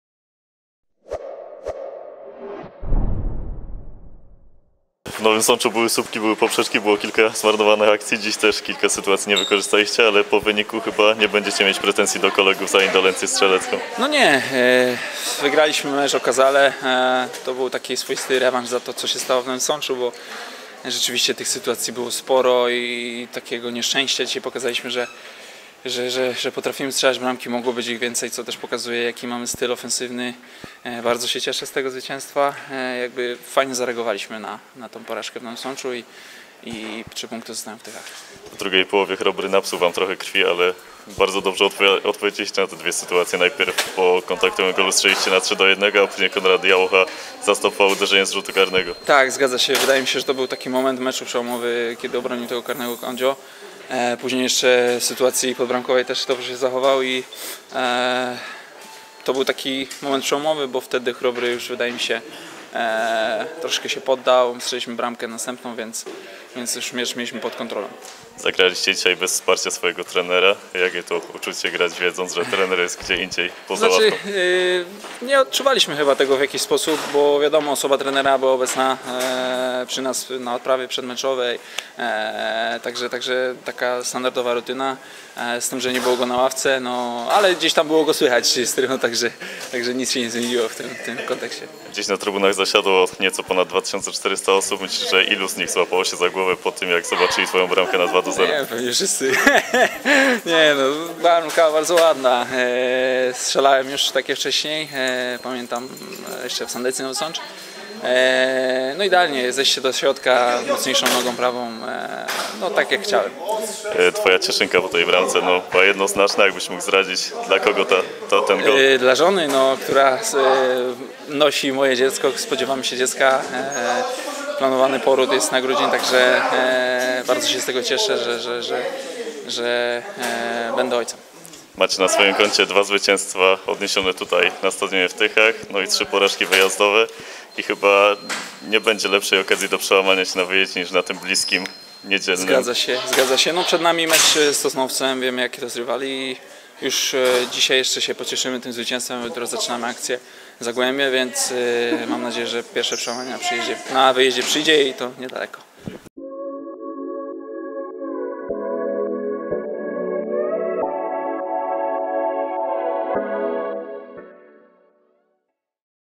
GKS Tychy - Chrobry Głogów 5:1. Bartosz Szeliga: "Po moim golu pewnie wszyscy złapali się za głowę"
Za GKS-em Tychy dwa mecze w roli gospodarza w sezonie 2019/20 w Fortuna 1. Lidze. Po efektownym triumfie nad Odrą Opole (4:1), dziś Tyszanie jeszcze efektowniej wygrali z Chrobrym Głogów. Ozdobą meczu był gol na 2:0 autorstwa Bartosza Szeligi. Piłkarz zadedykował go swojej żonie. Posłuchajcie, co miał do powiedzenia po spotkaniu.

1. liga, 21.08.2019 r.
GKS Tychy - Chrobry Głogów 5:1 (2:0)
1:0 Połap 24'
2:0 Szeliga 43'
2:1 Kowalczyk 56'
3:1 Biernat 57'
4:1 Szumilas 72'
5:1 Kasprzyk 90'